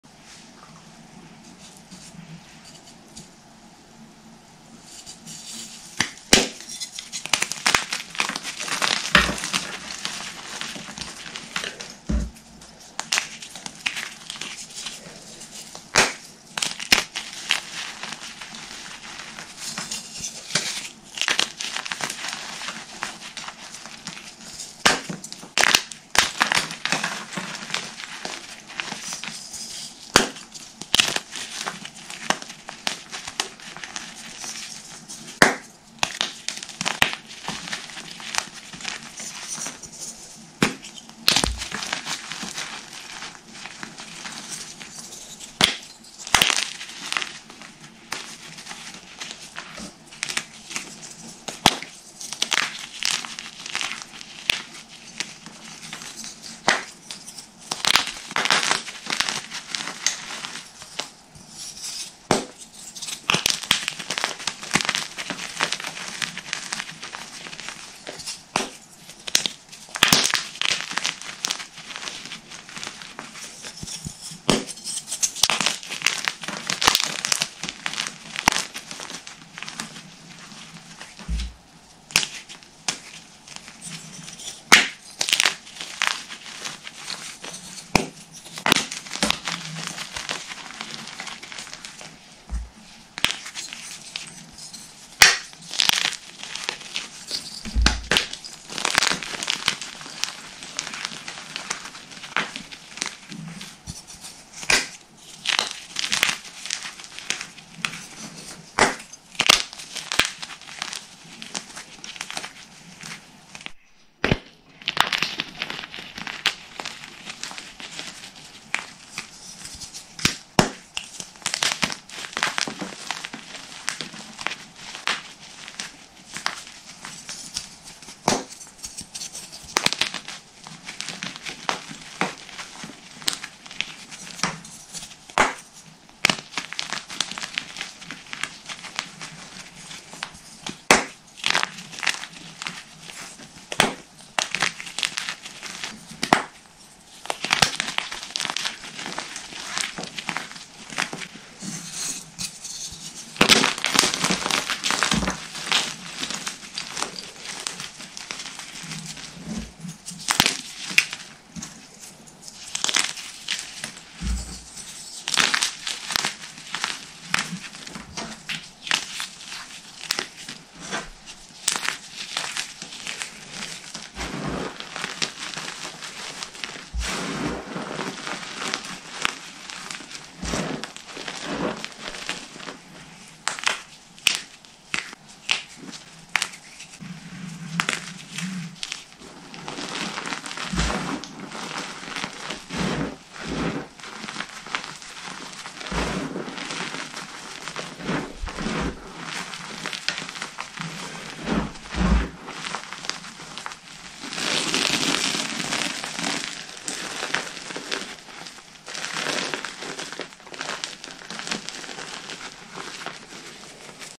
"PRETTY IN PINK" CRISPY DYED GYM CHALK
Pretty in pink.. Dyed gym chalk blocks with Sprinkles of glitter. With tiny Crispy bits. Super satisfying 💕 4th crush us full block crush. I've also edited this and put it into a short. So please go over and check that out on my feed ❤️

Want to see a short of this video with just a full crush and crispness??